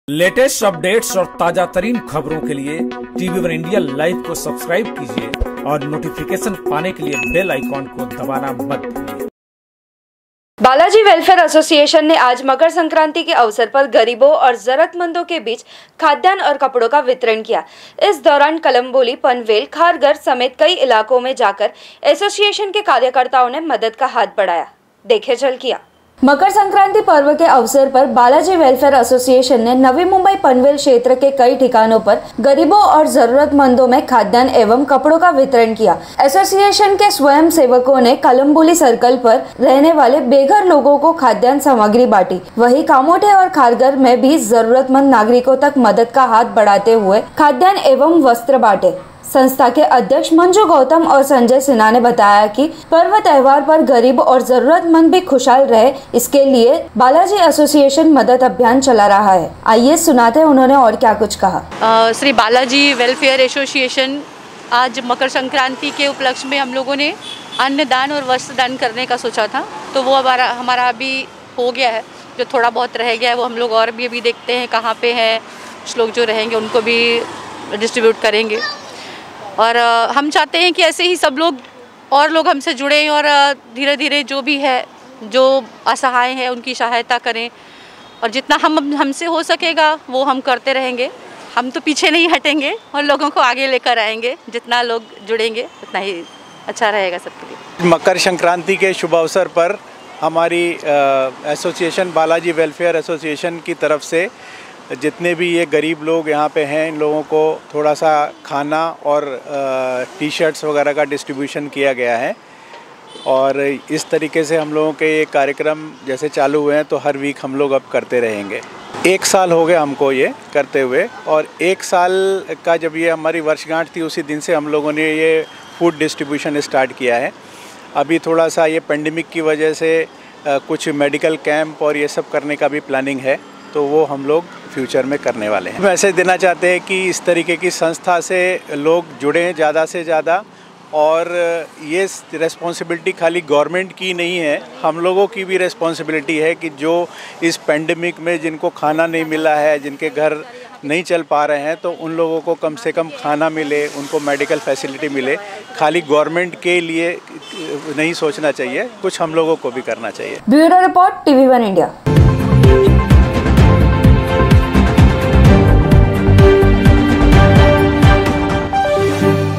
Balaji Association की मानवसेवी पहल: मकर संक्रांति पर गरीबों में बांटे कपड़े और खाद्यान्न
बालाजी वेल्फेयर एसोसिएशन ने आज मकरसंक्रांति के अवसर पर गरीबों और जरूरतमंदों के बीच खाद्यान्न और कपड़ों का वितरण किया. इस दौरान कलंबोली, पनवेल, खारघर समेत कई इलाकों में जाकर एसोसिएशन के कार्यकर्ताओं ने मदद का हाथ बढ़ाया.